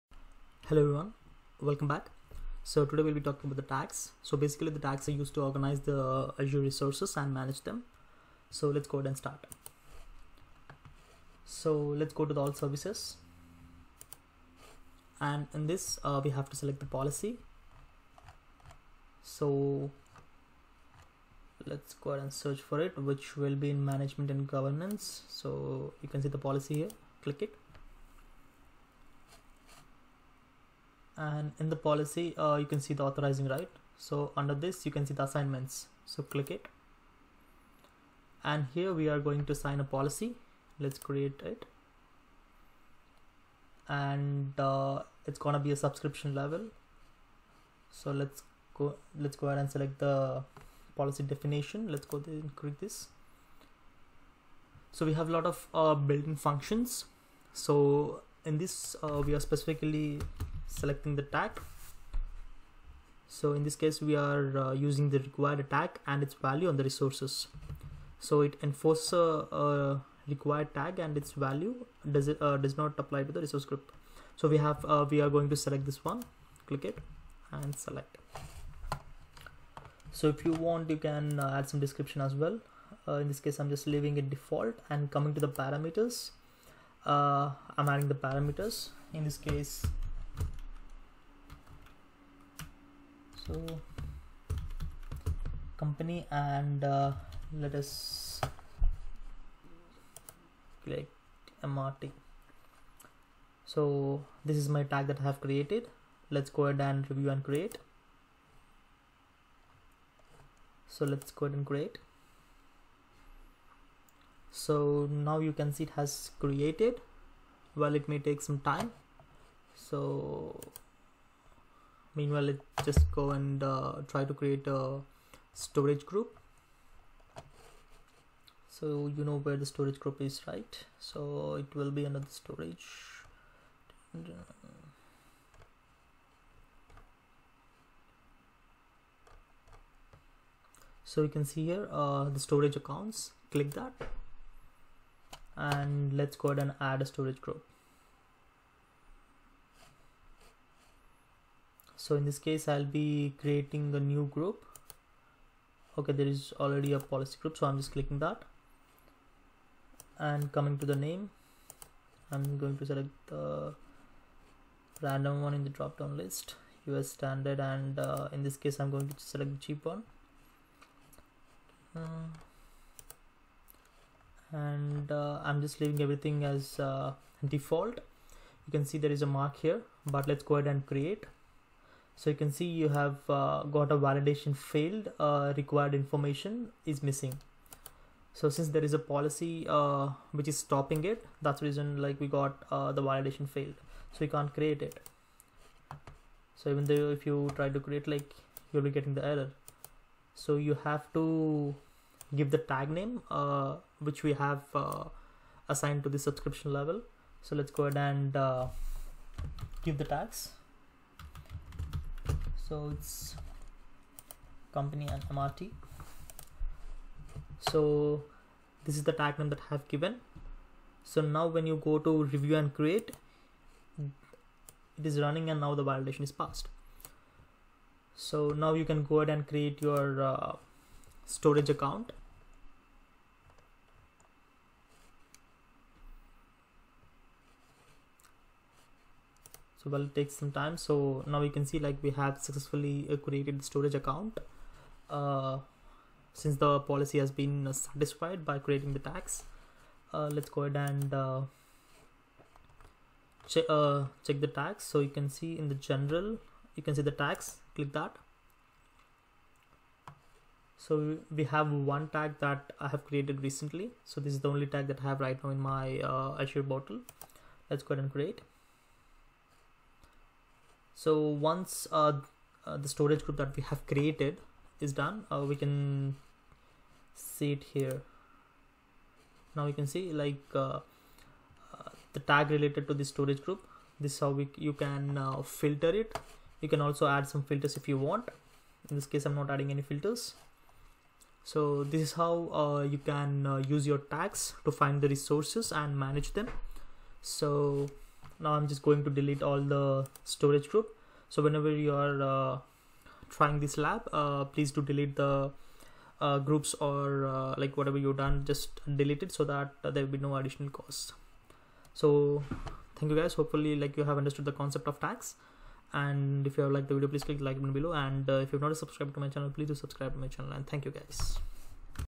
How to use Tags in Azure|| How to Assign Policy ?||Azure tutorial||AZ900
In this Video we are going to use a build in policy and assign it to the subscription level and test it by creating a storage group.

You apply tags to your Azure resources, resource groups, and subscriptions to logically organize them into a taxonomy. Each tag consists of a name and a value pair. For example, you can apply the name Environment and the value Production to all the resources in production.

Azure Policy helps to enforce organizational standards and to assess compliance at-scale. Through its compliance dashboard, it provides an aggregated view to evaluate the overall state of the environment, with the ability to drill down to the per-resource, per-policy granularity. It also helps to bring your resources to compliance through bulk remediation for existing resources and automatic remediation for new resources.

Common use cases for Azure Policy include implementing governance for resource consistency, regulatory compliance, security, cost, and management. Policy definitions for these common use cases are already available in your Azure environment as built-ins to help you get started.

You can also learn from Microsoft labs in Github.